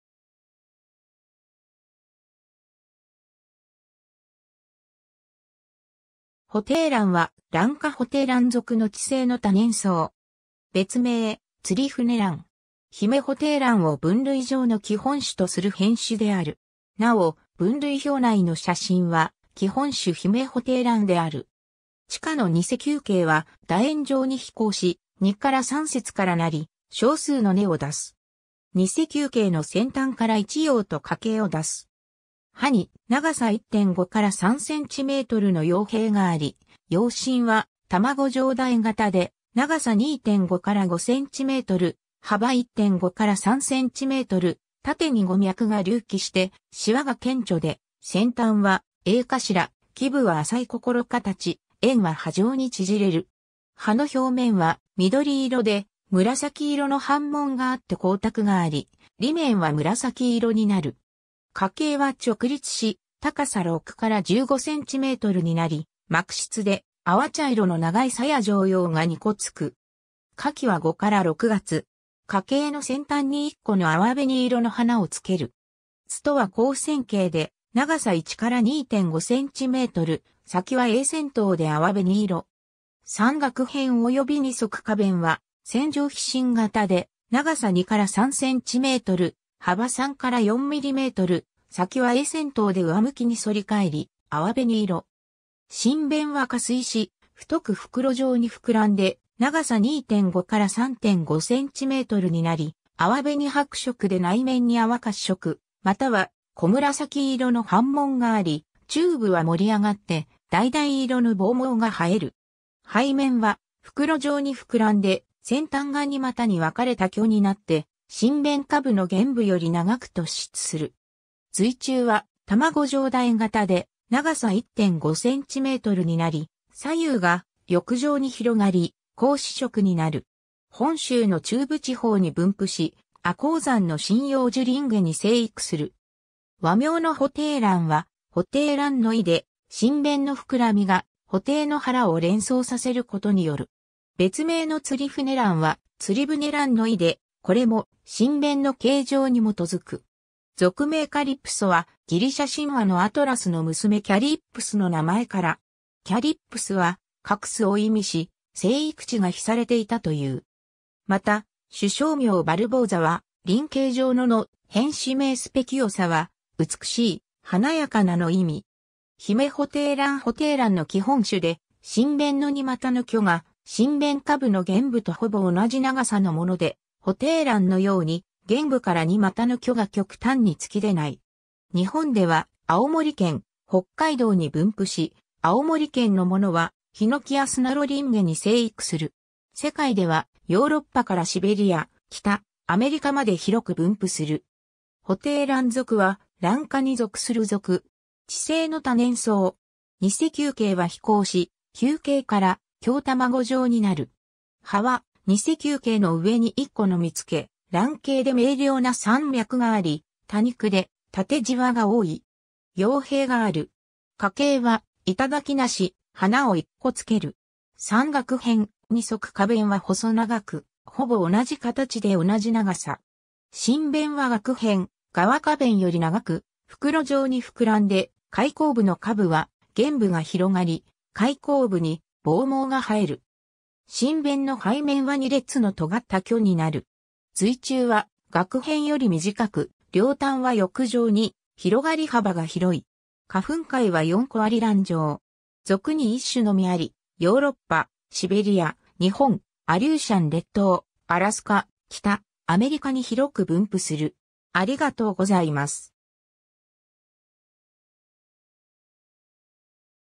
ホテイラン
ホテイラン（布袋蘭、学名：Calypso bulbosa var. speciosa）は、ラン科ホテイラン属の地生の多年草。
別名、ツリフネラン（釣舟蘭）。
 ヒメホテイラン（姫布袋蘭、学名：Calypso bulbosa）を分類上の基本種とする変種である。
なお、分類表内の写真は、基本種ヒメホテイランである。
 地下の偽球茎は楕円状に肥厚し、2-3節からなり、少数の根を出す。
偽球茎の先端から1葉と花茎を出す。
葉に長さ1.5-3cmの葉柄があり、葉身は卵状楕円形で、長さ2.5-5cm、幅1.5-3cm、縦に5脈が隆起してしわが顕著で、先端は鋭頭、基部は浅い心形、縁は波状に縮れる。
葉の表面は緑色で紫色の斑紋があって光沢があり、裏面は紫色になる。
花茎は直立し、高さ6-15cmになり、膜質で淡茶色の長い鞘状葉が2個つく。
花茎の先端に1個の淡紅色の花をつける。
苞は広線形で長さ1-2.5cm、先は鋭尖頭で淡紅色。
3萼片および2側花弁は線状披針形で、長さ2-3cm、幅3-4mm、先は鋭尖頭で上向きに反り返り、淡紅色。
唇弁は下垂し、太く袋状にふくらんで長さ2.5-3.5cmになり、淡紅白色で内面に淡褐色または濃紫色の斑紋があり、中部は盛り上がって橙色の房毛が生える。
背面は袋状にふくらんで、先端が2又に分かれた距になって、唇弁下部の舷部より長く突出する。
蕊柱は卵状楕円形で長さ1.5cmになり、左右が翼状に広がり、紅紫色になる。
 本州の中部地方に分布し、亜高山の針葉樹林下に生育する。
 和名のホテイランは、「布袋蘭」の意で、唇弁のふくらみが布袋の腹を連想させることによる。
別名のツリフネランは、「釣舟蘭」の意で、これも唇弁の形状に基づく。
 属名 Calypso は、ギリシャ神話のアトラスの娘 Kalypso の名前から。
kalypso は、「隠す」を意味し、生育地が秘されていたという。
また、種小名（種形容語）bulbosa は、「鱗茎状の」の、変種名 speciosaは、「美しい」「華やかな」の意味。
 （2017年、環境省） ヒメホテイラン（姫布袋蘭、学名：Calypso bulbosa (L.) Oakes var. bulbosa） - ホテイランの基本種で、唇弁の2又の距が、唇弁下部の舷部とほぼ同じ長さのもので、ホテイランのように舷部から2又の距が極端に突き出ない。
日本では、青森県、北海道に分布し、青森県のものはヒノキアスナロ林下に生育する。
世界ではヨーロッパからシベリア、北アメリカまで広く分布する。
 （2017年、環境省） ホテイラン属（ホテイランぞく、学名：Calypso Salisb.、和名漢字表記：布袋蘭属）は、ラン科に属する属。
地生の多年草。
偽球茎は肥厚し、球形から狭卵状になる。
葉は偽球茎の上に1個のみつけ、卵形で明瞭な3脈があり、多肉で縦じわが多い。
葉柄がある。
花茎は頂生し、花を1個つける。
3萼片、2側花弁は細長く、ほぼ同じ形で同じ長さ。
唇弁は萼片、側花弁より長く、袋状にふくらんで、開口部の下部は舷部が広がり、開口部に房毛が生える。
唇弁の背面は2裂のとがった距になる。
蕊柱は萼片より短く、両端は翼状に広がり幅が広い。
花粉塊は4個あり卵状。
属に1種のみあり、ヨーロッパ、シベリア、日本、アリューシャン列島、アラスカ、北アメリカに広く分布する。